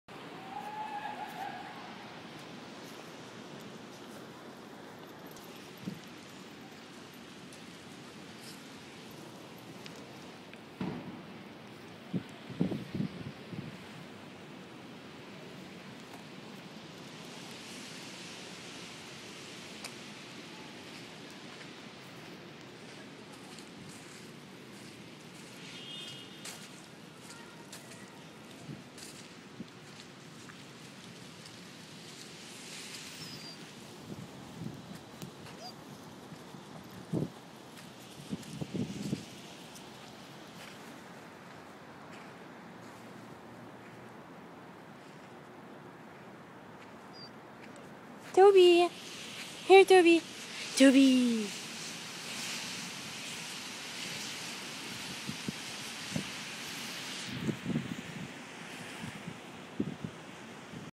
Tobby playing
Tobby following me. He hates the leash but I an trying to get him used to it. He loves te grass though.